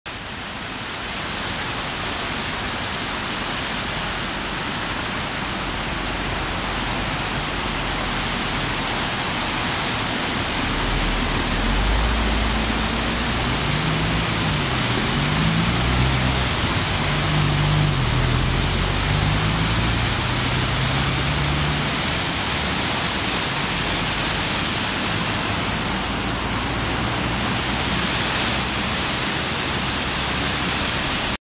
Exploring Olofström's Dam A Scenic Waterway Adventure
Join me as we visit Olofström's dam, a fascinating waterway nestled amidst nature's beauty. Discover the serene surroundings and the gentle flow of water. Immerse yourself in the tranquility of the landscapes. Whether you're an architecture enthusiast or simply seeking a peaceful retreat, Olofström's dam has something special to offer. 🌊🏞️🚶‍♂️